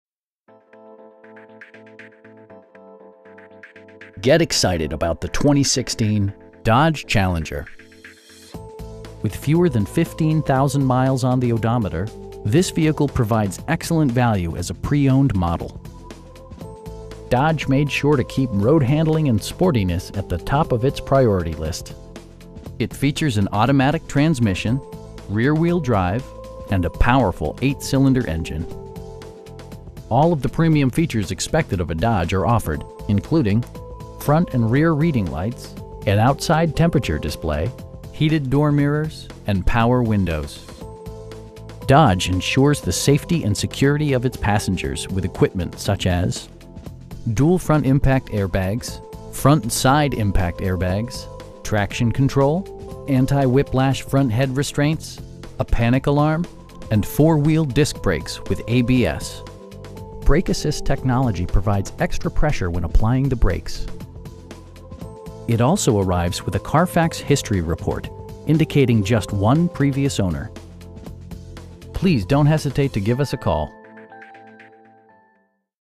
2016 Dodge Challenger R/T in Norfolk, VA 23518
Get excited about the 2016 Dodge Challenger. With fewer than 15,000 miles on the odometer, this vehicle provides excellent value as a pre-owned model. Dodge made sure to keep road-handling and sportiness at the top of it's priority list. It features an automatic transmission, rear-wheel drive, and a powerful 8 cylinder engine. All of the premium features expected of a Dodge are offered, including: front and rear reading lights, an outside temperature display, front bucket seats, telescoping steering wheel, heated door mirrors, an overhead console, and power windows. Dodge ensures the safety and security of its passengers with equipment such as: dual front impact airbags, front side impact airbags, traction control, anti-whiplash front head restraints, a panic alarm, and 4 wheel disc brakes with ABS. Brake assist technology provides extra pressure when applying the brakes. It also arrives with a Carfax history report, indicating just one previous owner. You will have a pleasant shopping experience that is fun, informative, and never high pressured. Please don't hesitate to give us a call.